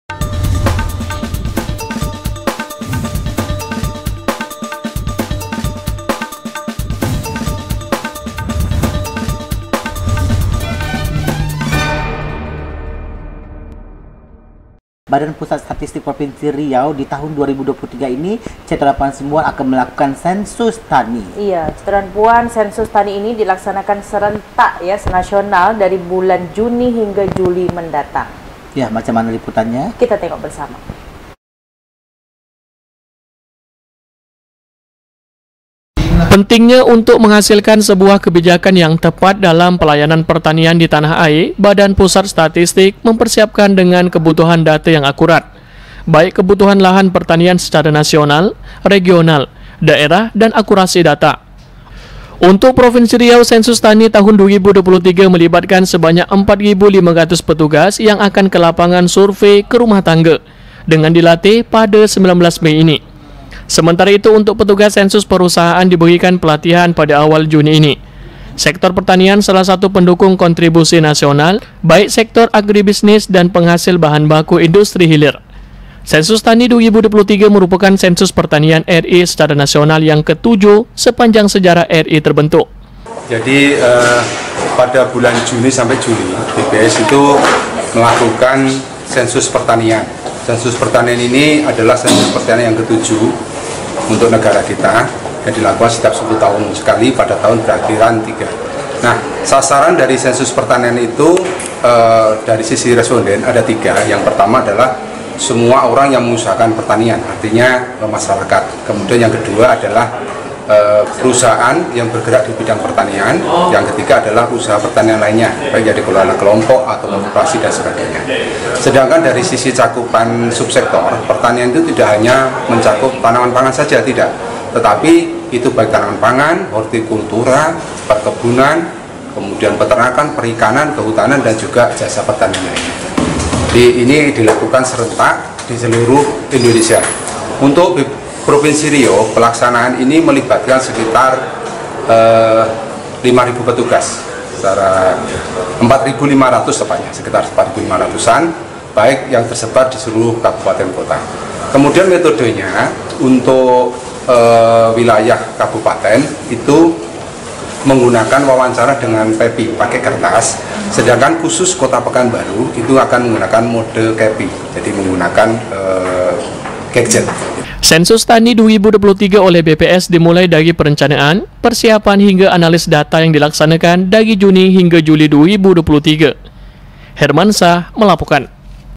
SENSUS TANI DILAKSANAKAN JUNI JULI 2023
Pekanbaru, riautelevisi.com- Badan Pusat Statistik, Provinsi Riau, akan melaksanakan Sensus Tani 2023. Sensus tani ini dilaksanakan serentak secara nasional pada Juni-Juli mendatang.

Pentingnya  untuk menghasilkan sebuah kebijakan yang tepat dalam pelayanan pertanian di tanah air, Badan Pusat Statistik mempersiapkan dengan kebutuhan  data yang akurat,  baik kebutuhan lahan pertanian secara nasional, regional, daerah, dan akurasi data.

Untuk Provinsi Riau, Sensus Tani tahun 2023, melibatkan sebanyak  4500 petugas yang akan  lapangan survei ke rumah tangga, dengan dilatih pada 19 Mei ini. Sementara itu, untuk petugas sensus perusahaan, diberikan pelatihan pada awal Juni ini.

Sektor pertanian salah satu pendukung  kontribusi naisonal, baik sektor agribisnis dan   penghasil bahan baku industri hilir. Sensus Tani 2023 merupakan sensus pertanian Republik Indonesia secara nasional yang ke tujuh sepanjang sejarah Republik Indonesia terbentuk.

Sensus Tani 2023 oleh BPS, dimulai dari perencanaan, persiapan hingga analisis data yang dilaksanakan dari Juni hingga Juli 2023.  (H)